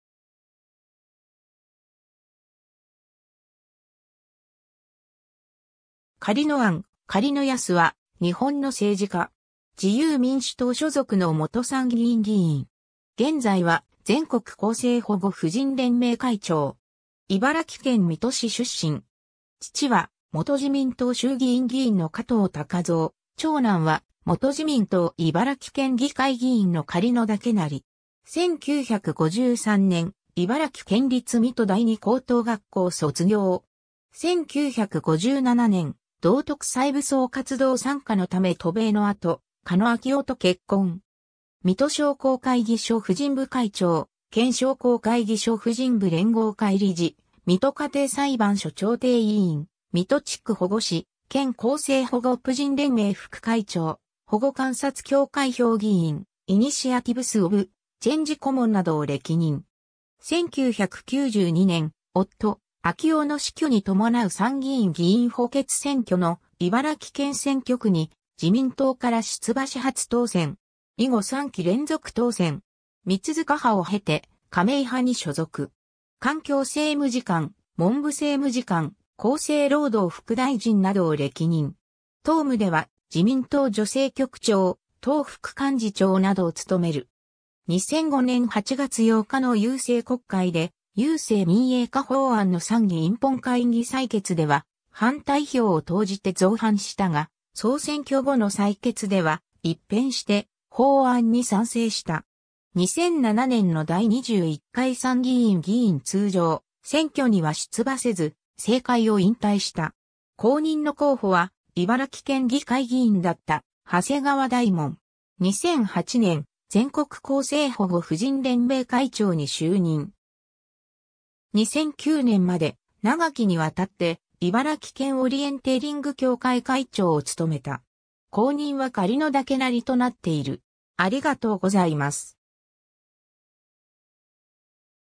狩野安
狩野安（厚生労働副大臣就任時） 狩野 安（かのう やす、1935年3月5日 - ）は、日本の政治家。
自由民主党所属の元参議院議員（3期）。
現在は全国更生保護婦人連盟会長。
茨城県水戸市出身。
 父は元自民党衆議院議員の加藤高蔵、長男は元自民党茨城県議会議員の狩野岳也。
 1953年、茨城県立水戸第二高等学校卒業。
1957年、道徳再武装（MRA）活動参加のため渡米の後、狩野明男と結婚。
水戸商工会議所婦人部会長・県商工会議所婦人部連合会理事・水戸家庭裁判所調停委員・水戸地区保護司・県更生保護婦人連盟副会長・保護観察協会評議員・イニシアティブス・オブ・チェンジ顧問などを歴任。
 1992年、夫・明男の死去に伴う参議院議員補欠選挙の茨城県選挙区に自民党から出馬し初当選。
以後3期連続当選。
三塚派を経て、亀井派に所属。
環境政務次官・文部政務次官・厚生労働副大臣（雇用対策等の労働分野や少子化対策の担当）などを歴任。
党務では自民党女性局長・党副幹事長などを務める。
 2005年8月8日の郵政国会で郵政民営化法案の参議院本会議採決では反対票を投じて造反したが、総選挙後の採決では、一変して法案に賛成した。
 2007年の第21回参議院議員通常選挙には出馬せず、政界を引退した。
後任の候補は、茨城県議会議員だった長谷川大紋。
 2008年、全国更生保護婦人連盟会長に就任。
 2009年まで、長きにわたって茨城県オリエンテーリング協会会長を務めた。
後任は狩野岳也となっている。